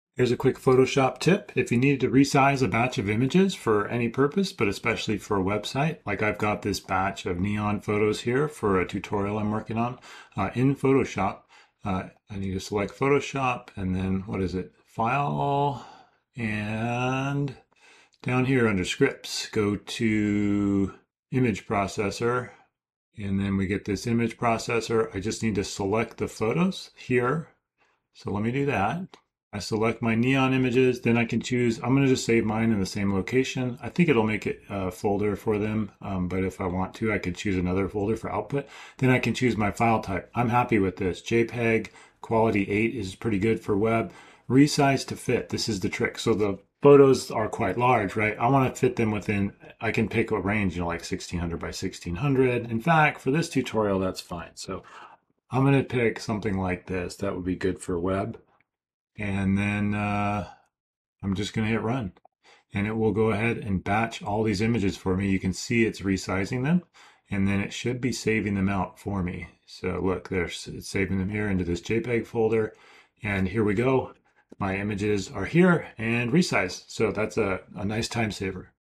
How to Batch Resize Images in Photoshop
A quick tutorial showing how to resize a batch of images in Photoshop. This can work for any size images, but can be especially handy for resizing website images or photography work.